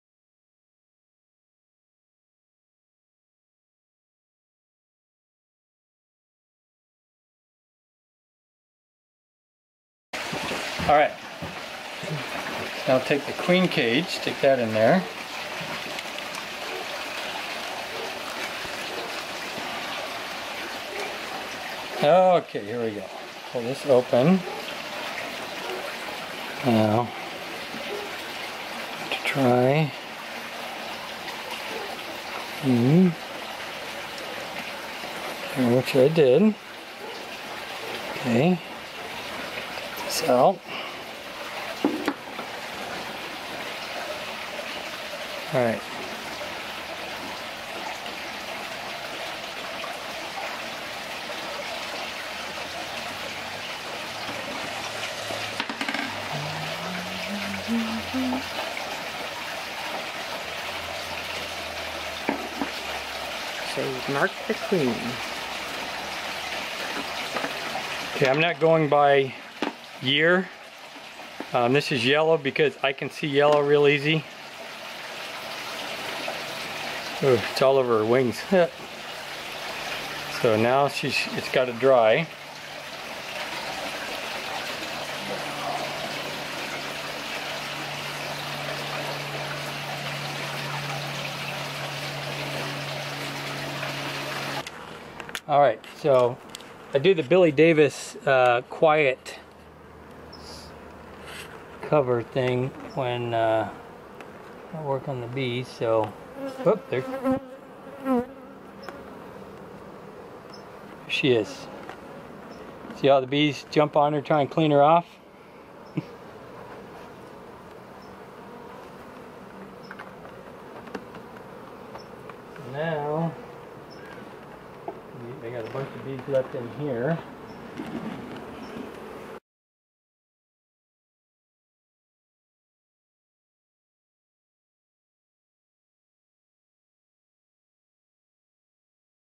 Queen bee marking
This was my first attempt at marking a queen.  The queen was from a swarm that was captured.  The colony is doing well.  I have moved it into a 10 frame deep box.  At the rate it is growing and she is laying I will add another deep next week.